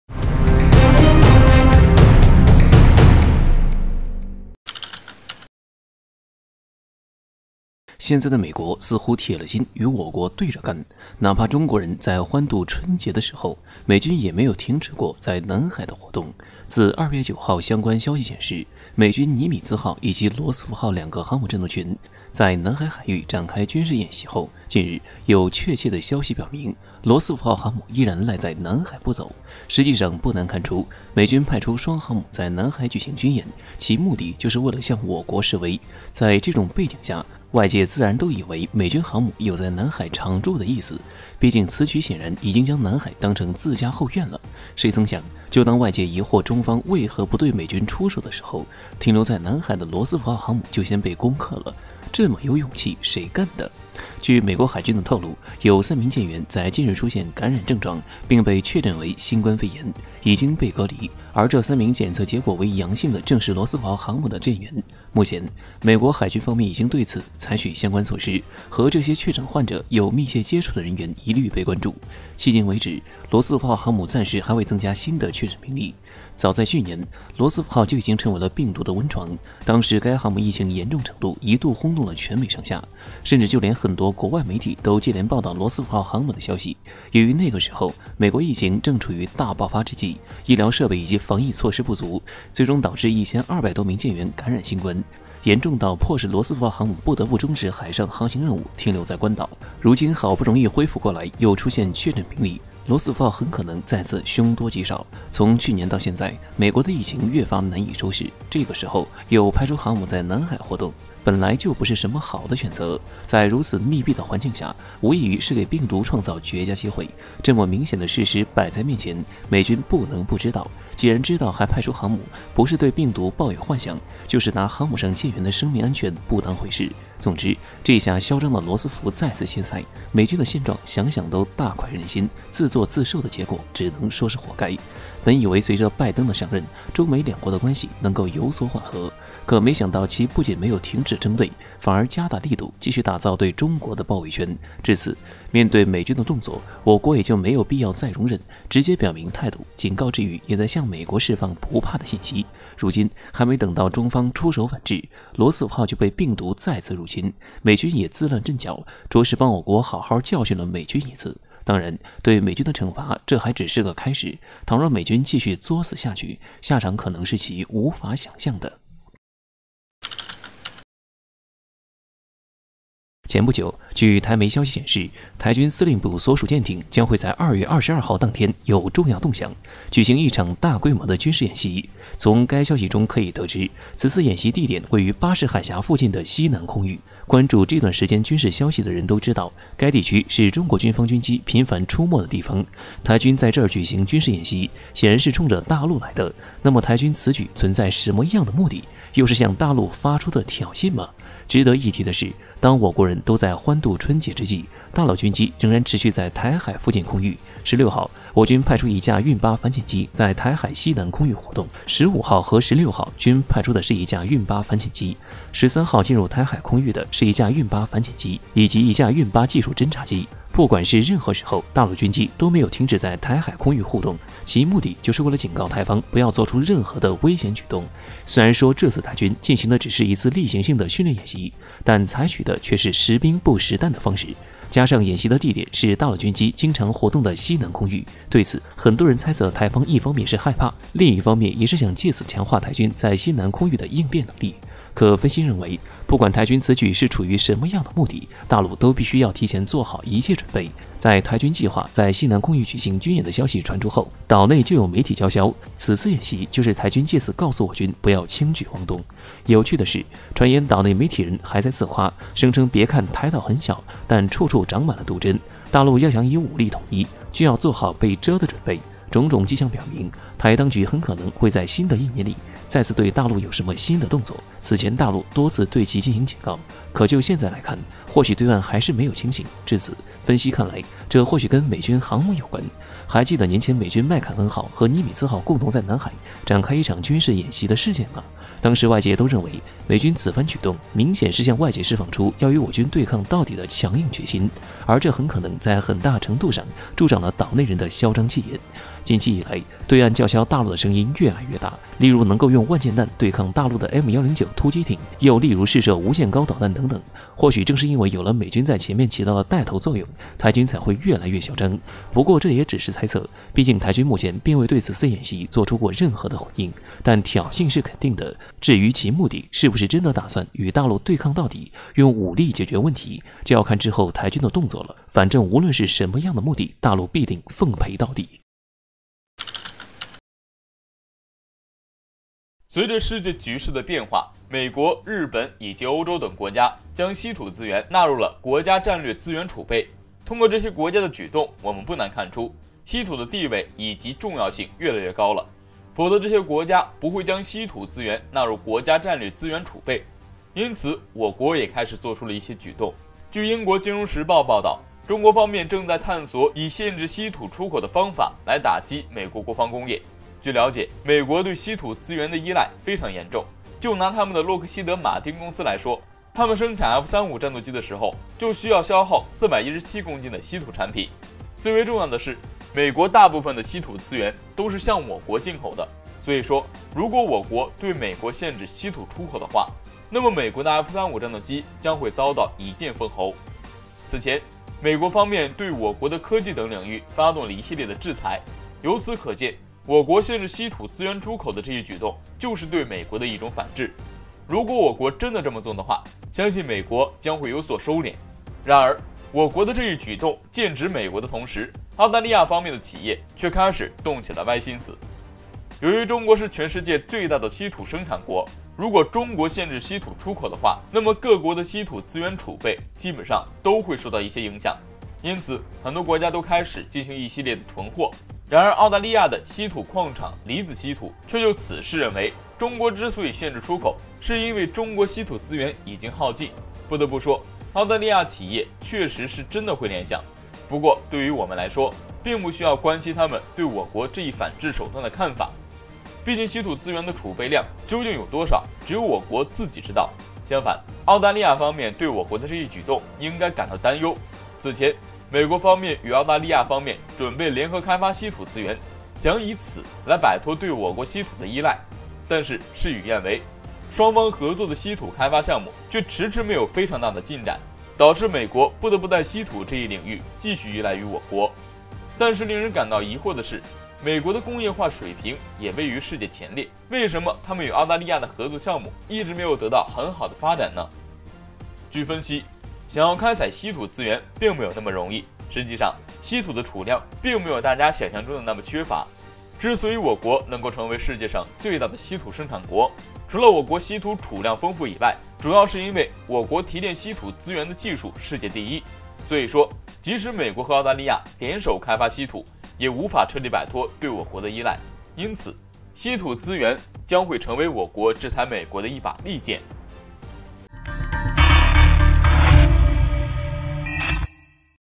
值得關注！稀土將成為我國反制美國的一把利劍，F-35或遭一劍封喉【強國軍事】
1、00:00-02:50 “羅斯福”賴在南海不走，突遭重擊，誰幹的？中方還沒來得及出手
2、02:51-05:28 22日，台軍欲有重要動向，關美航母啥事？台媒：大陸做好被蟄準備
3、05:29-08:24 值得關注！稀土將成為我國反制美國的一把利劍，F-35或遭一劍封喉
值得關注！稀土將成為我國反制美國的一把利劍，F-35或遭一劍封喉
话说国际形势，解说最新军事武器。
强国军事 稀土 我們的影片會遵循公平、尊重的原則，確保不會包含任何仇恨言論的元素。